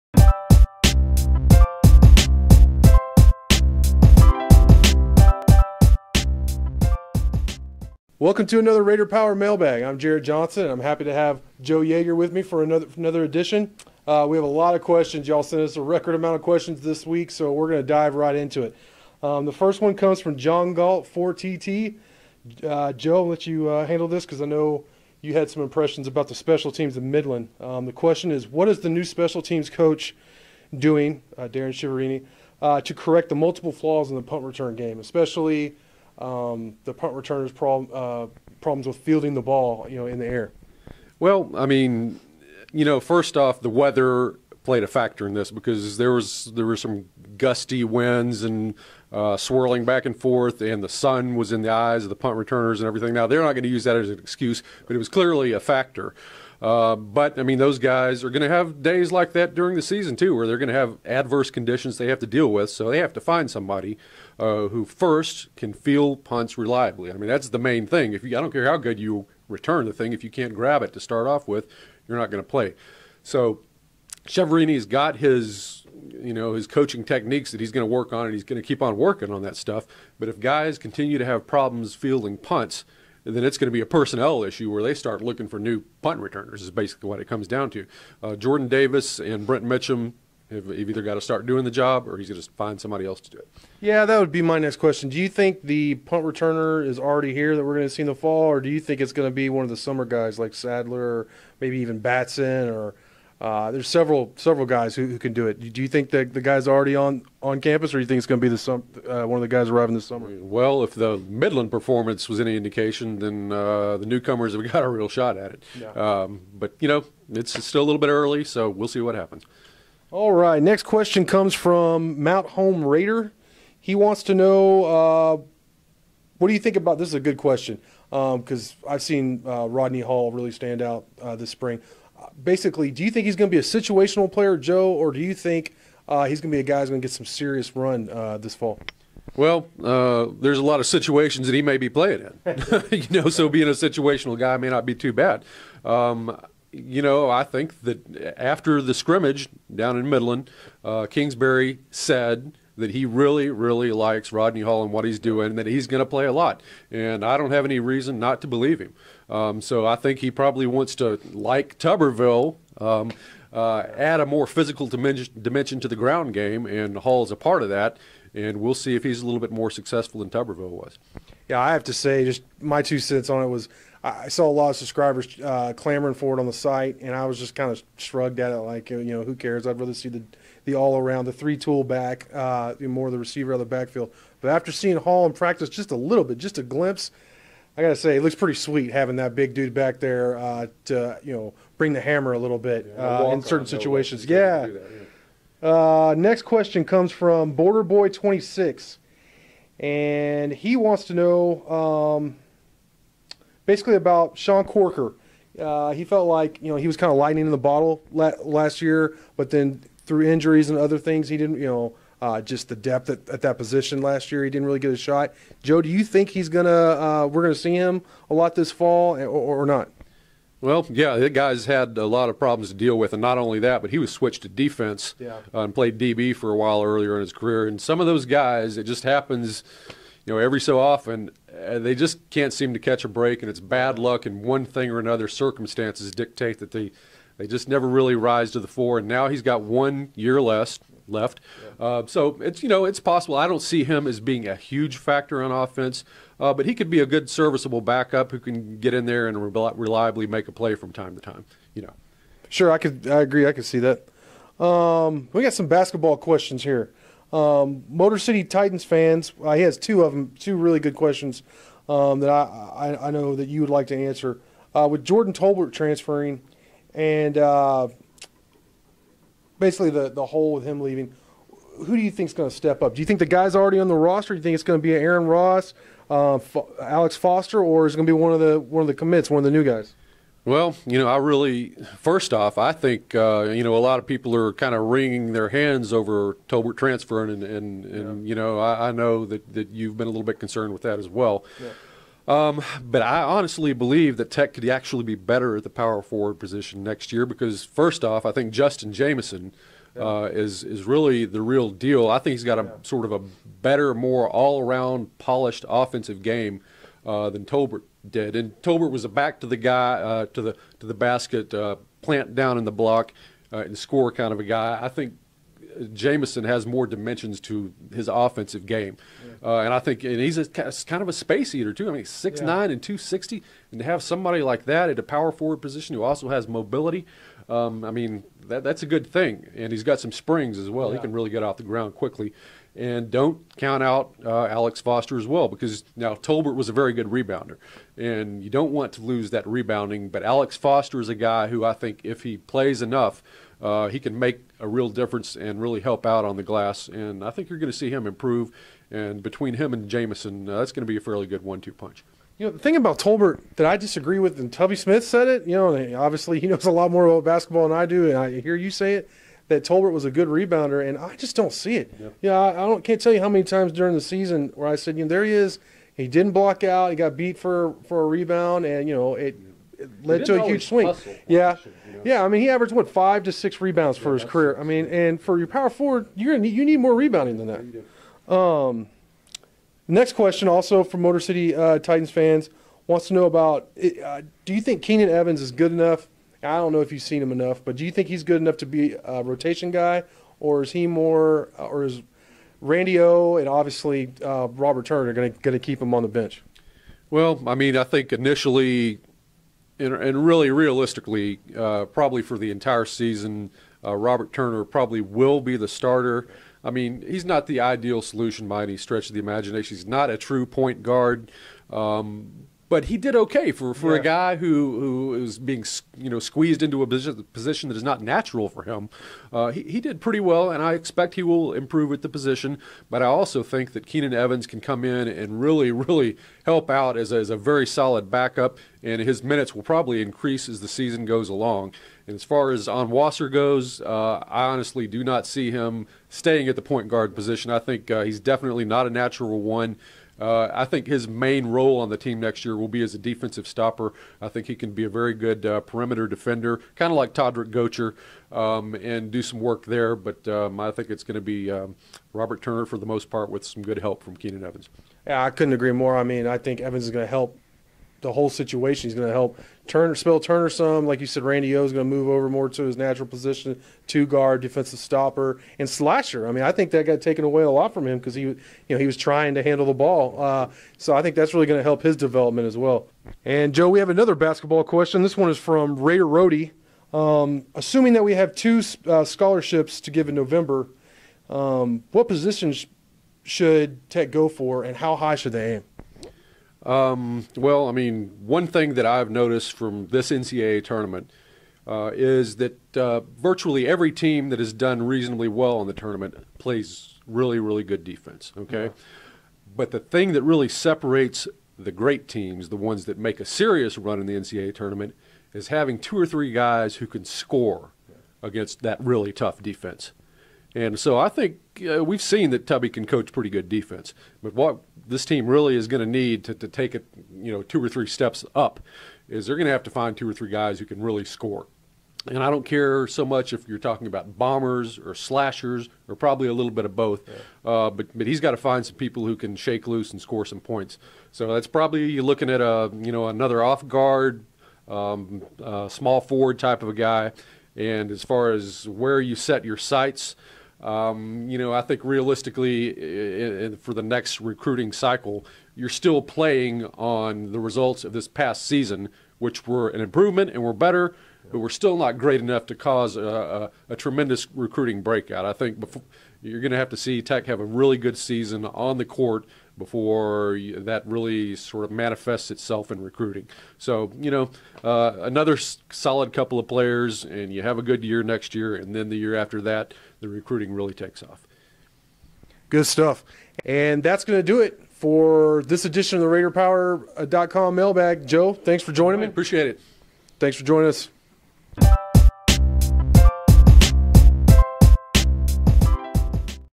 RaiderPower Mailbag 04-04-2014
Joe Yeager and Jarret Johnson answer question from RaiderPower.com subscribers about Texas Tech football and hoops.